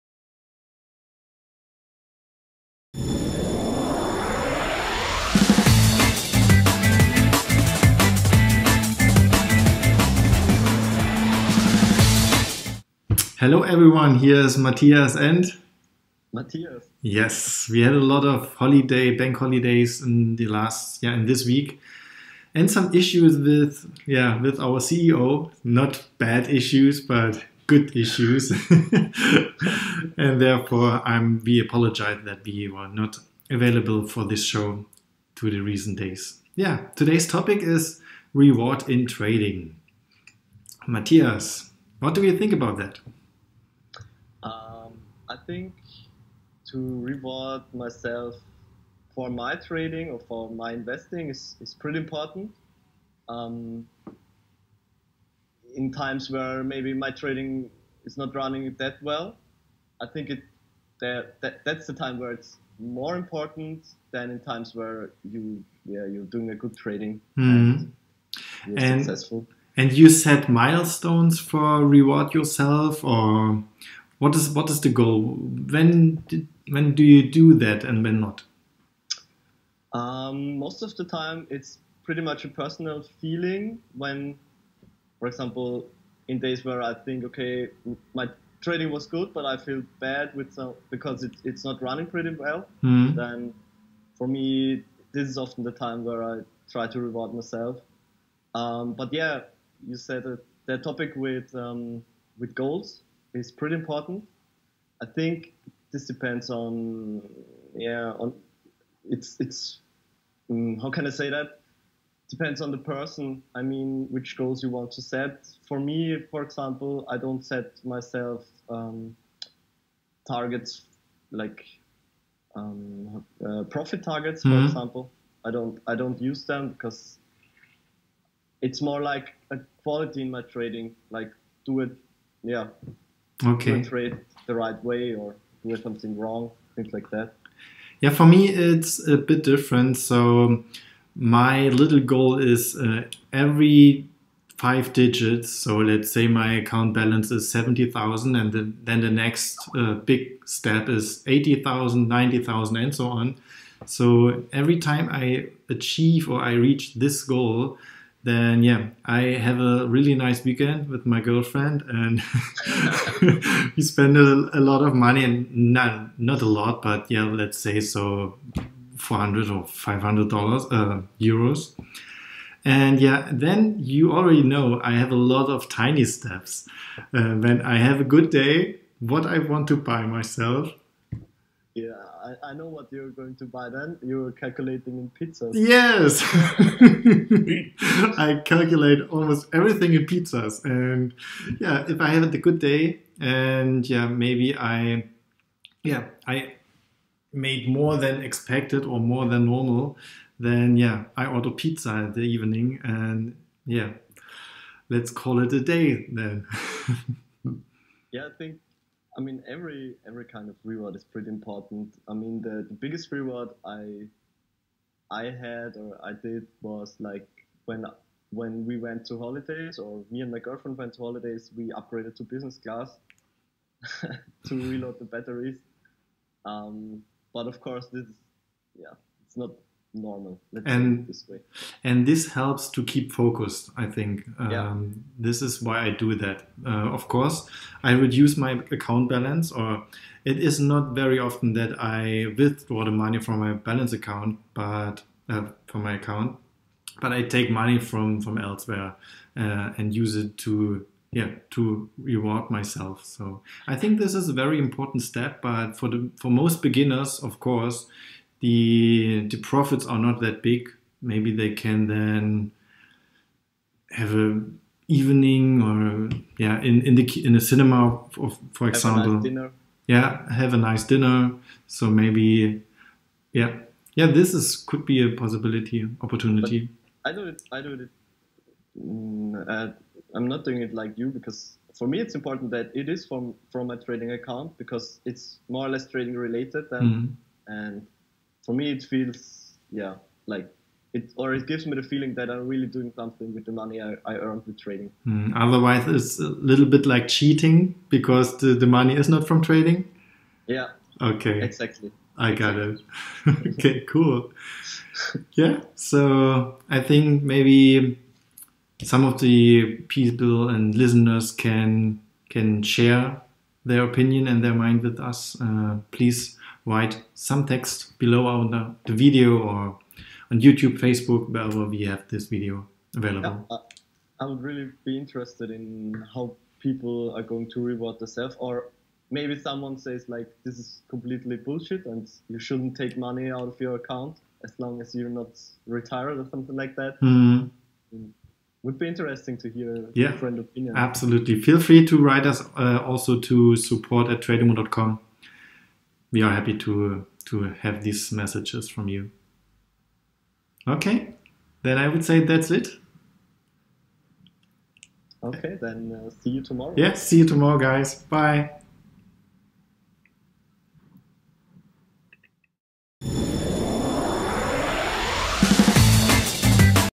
Reward in Trading, important or completely unnecessary? (TTS Ep. 35) | Tradimo
Do you reward yourself for your trading? What is your opinion about this topic? Do you buy things for you to stay on track or is it unnecessary?